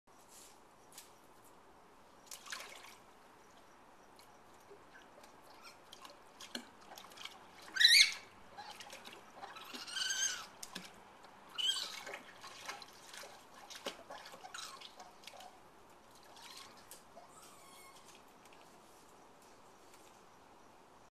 Short sound bite of baby raccoon in swimming pool in the night
This video follows up the earlier daytime video where baby raccoon was very quiet as it was tossed around like a wet rag. This is very short. As soon as they sense I am nearby, the sound & swimming stops. They have very keen hearing & sense of awareness.